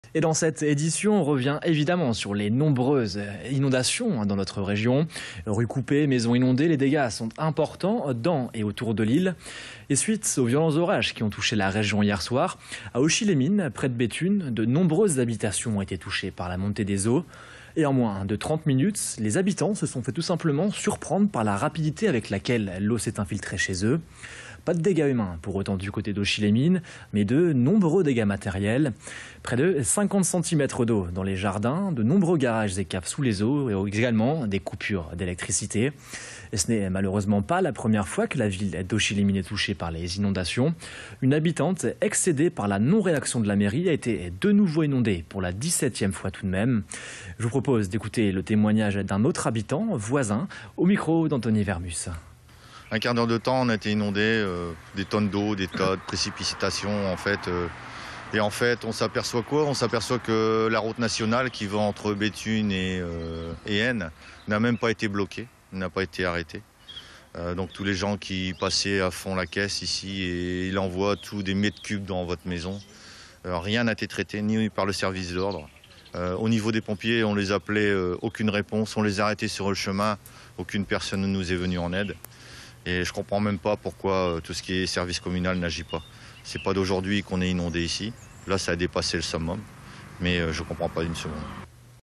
Inondations à Auchy-Les-Mines
La ville de Auchy-Les-Mines dans le Pas-de-Calais a été touchée par la montée des eaux.

Sites webs/blogs, copiez cette vidéo avec le bouton "Intégrer" !